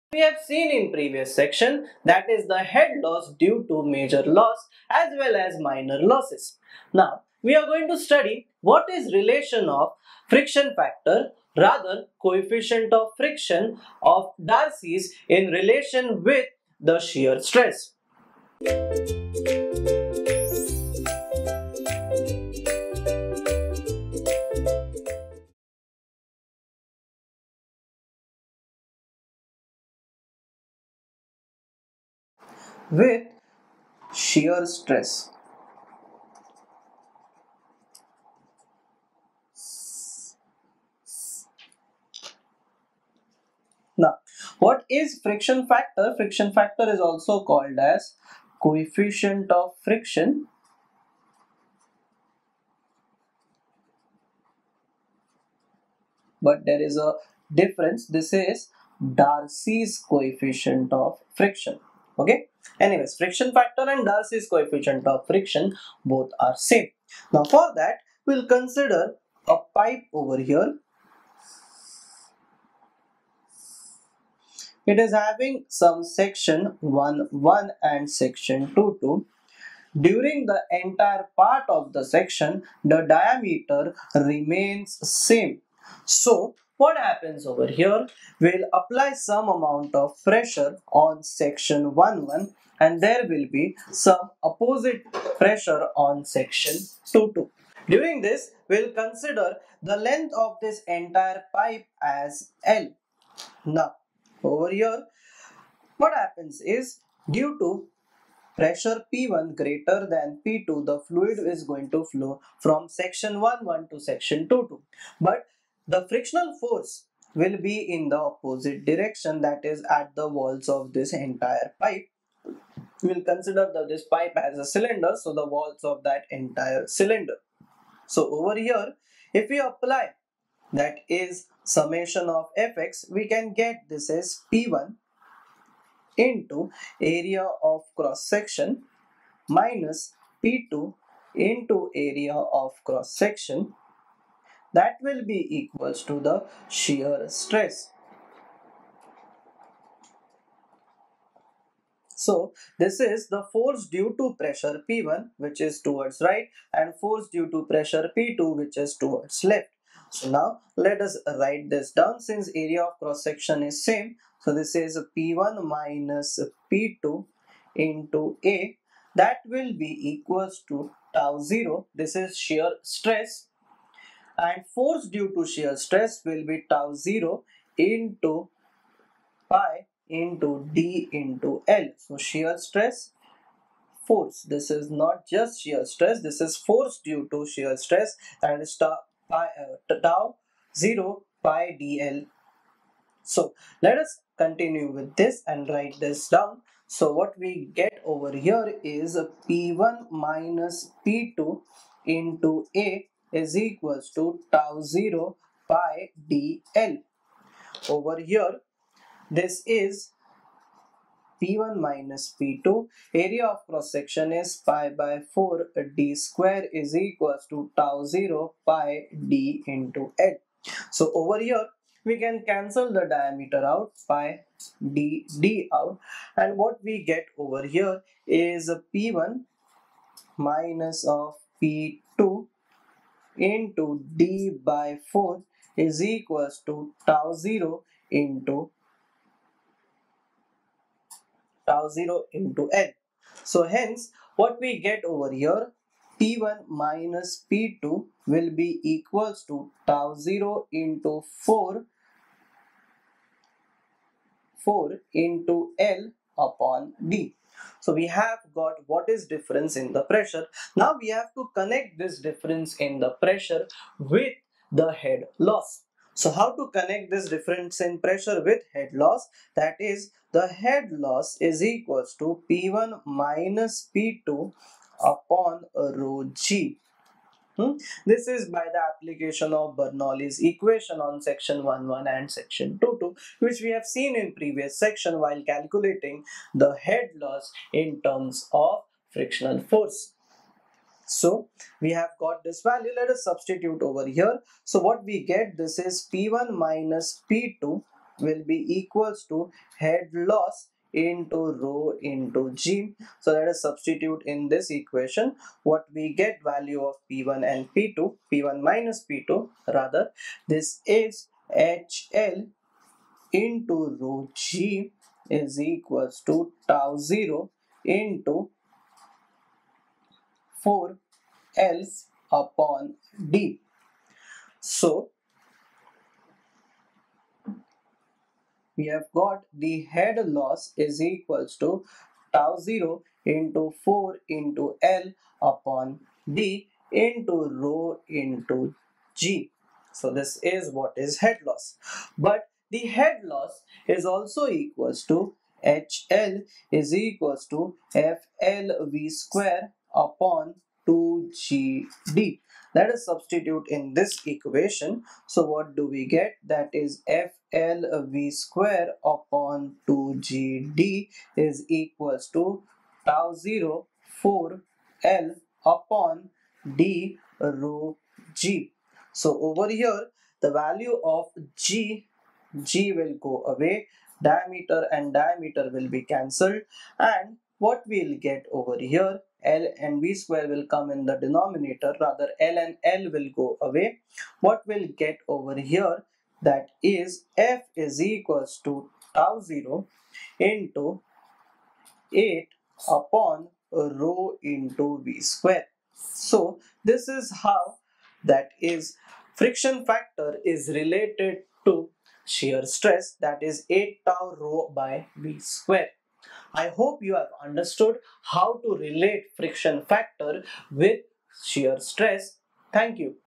Darcy Coefficient of Friction in Terms of Shear Stress - Real Fluid Flows - Fluid Mechanics 1
Video Name - Darcy Coefficient of Friction in Terms of Shear Stress

Chapter - Real Fluid Flows

Happy Learning!